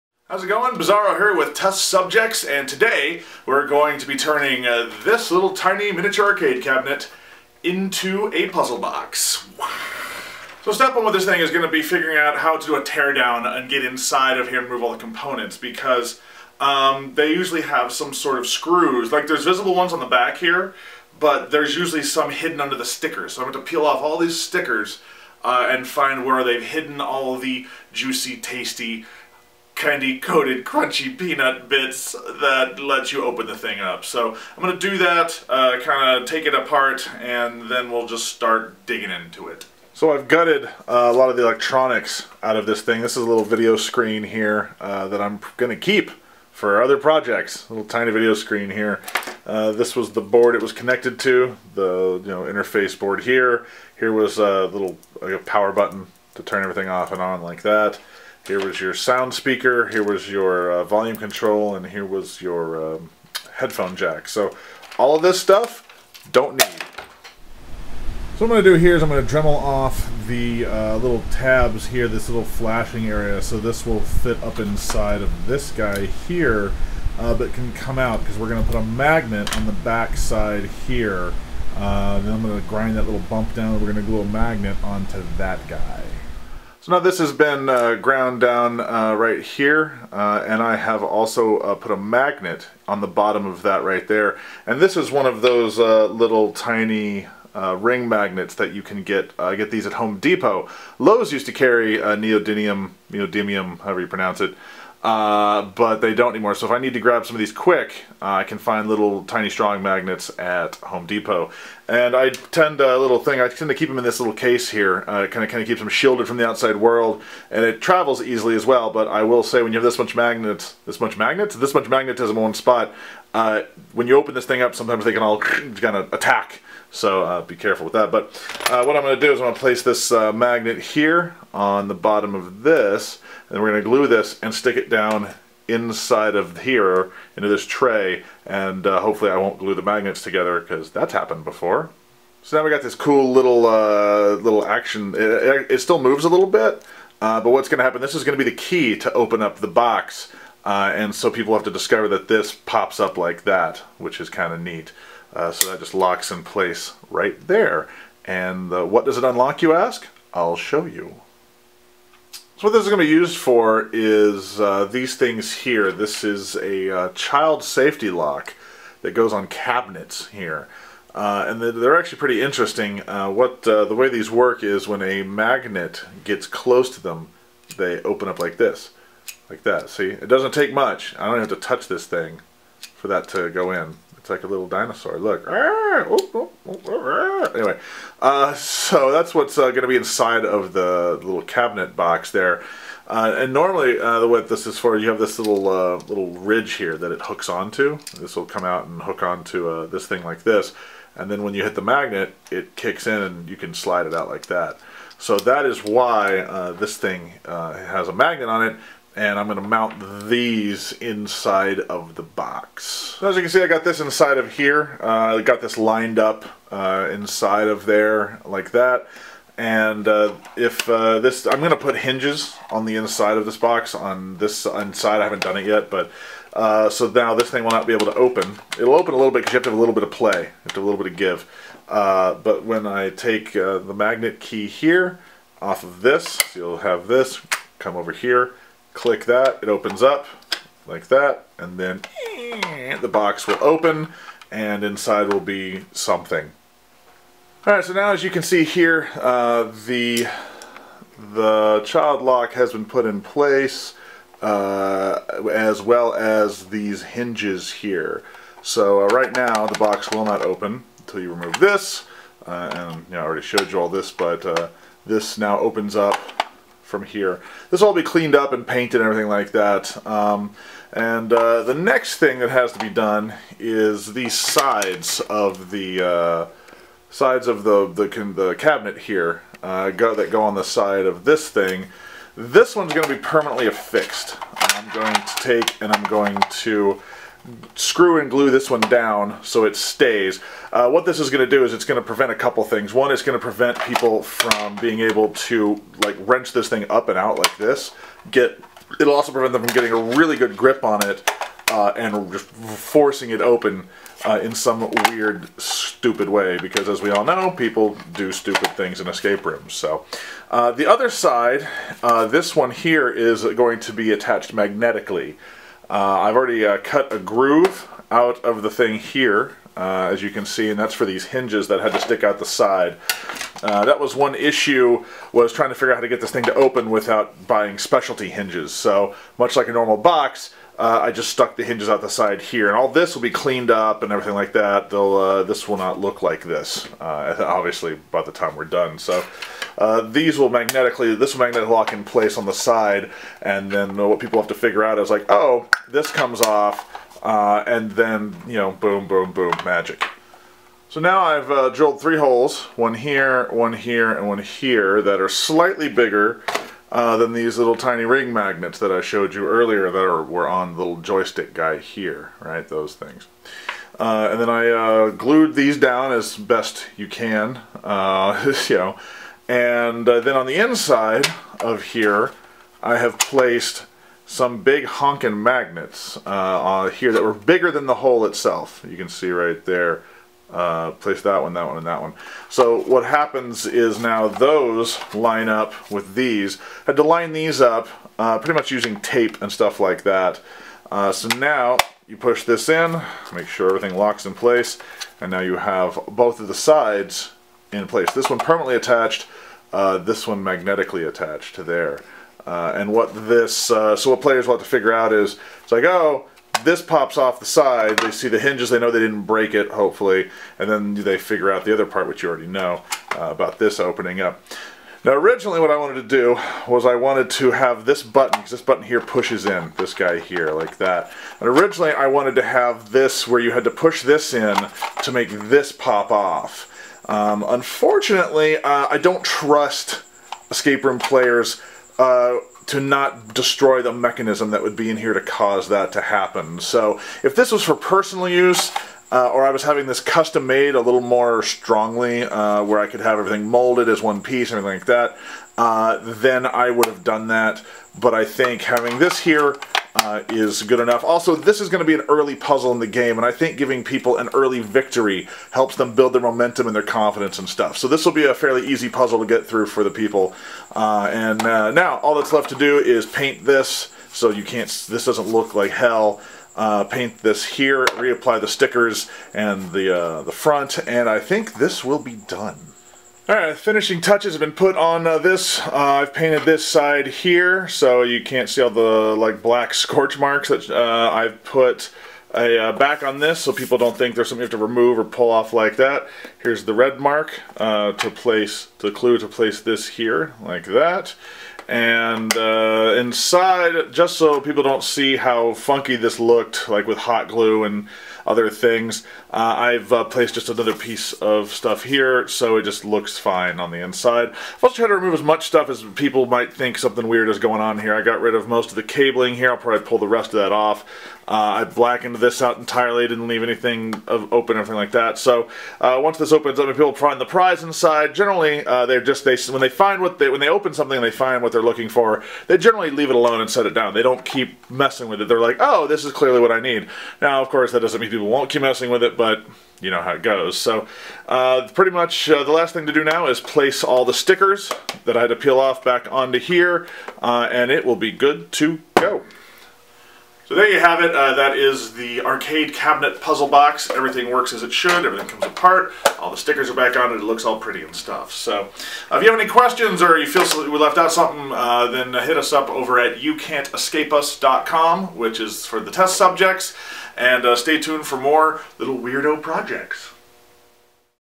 Arcade Puzzle Box
The Test Subjects unveil the creation of a puzzle box created from a tiny arcade machine.